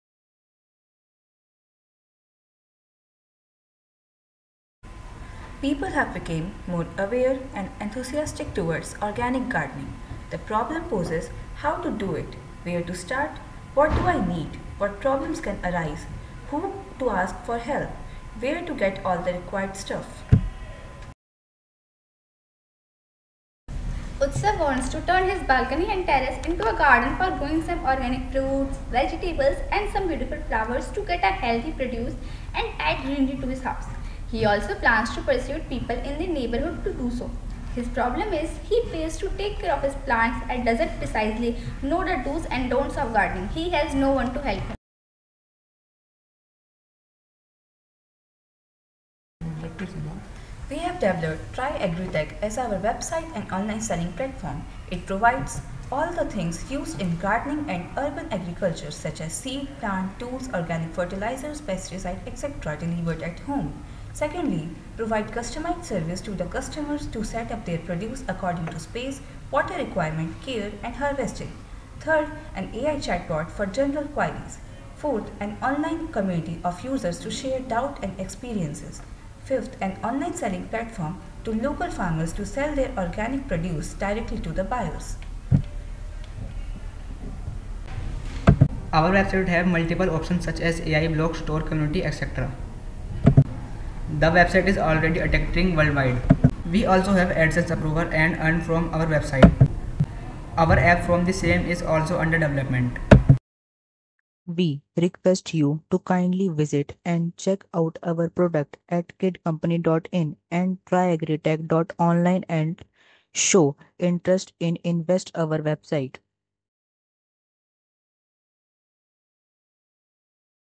UID - 2F27400 | Youth Ideathon 2024 | TOP 500 | Team - AGRO DEVELOPERS
Youth Ideathon 2024 | Students - Priyanshu Kumar | Jagriti Kumari | Abhinav Chauhyan | Devansh Dubey | Anshika Tiwari | Senior (9 - 12 Grade) | SWAMI VIVEKANANDA SARASWATI VIDYA MANDIR | Team- AGRO DEVELOPERS

-Farmer's and plant lover's friend..

-The best place to accelerate your love towards plants shop and sell now by gaining a lot of information, all at one place